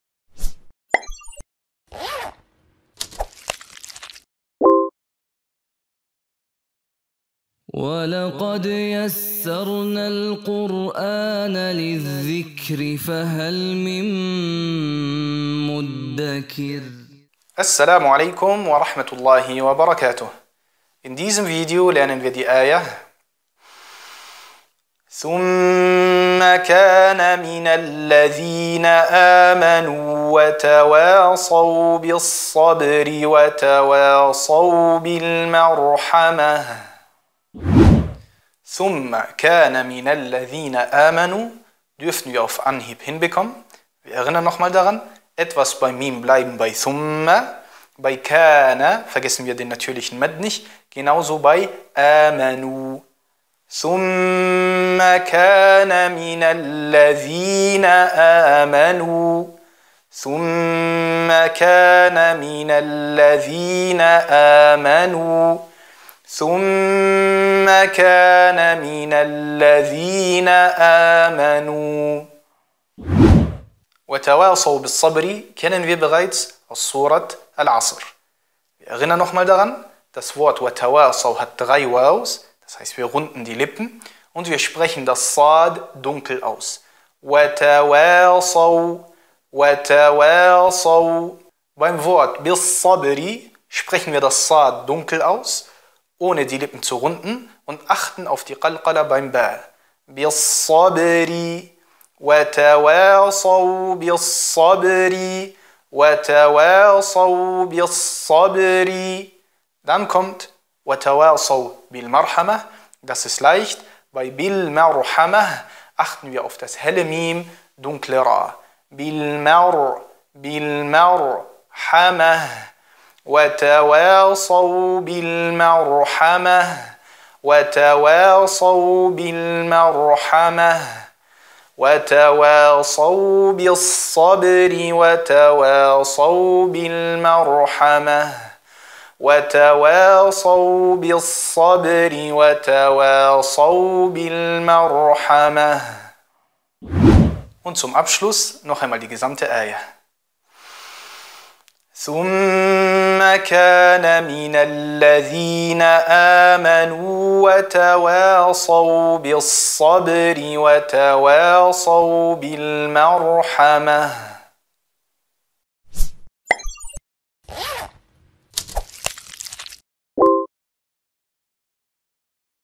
Surah Al-Balad Ayah 17 - Qur'ān lesen leicht gemacht | سورة البلد
Dieser Kurs ist für Teilnehmer mittlerer Wissensstufe, d.h. diejenigen, die bereits eigenständig den Qurʾān lesen können und  nun auswendig lernen oder zumindest mit richtigem Taǧwīd lesen wollen.

Wir beginnen mit der Al-Fātiḥah, dann mit den Kleinsūren und analysieren diese Buchstabe für Buchstabe, Wort für Wort, Āyah für Āyah.

In jeder Videoreihe analysieren wir eine Āyah auf ihre Taǧwīdregeln.
Das erleichtert dem Teilnehmer vor allem gleichzeitig mit auswendig zu lernen.

Du willst noch mehr sehen?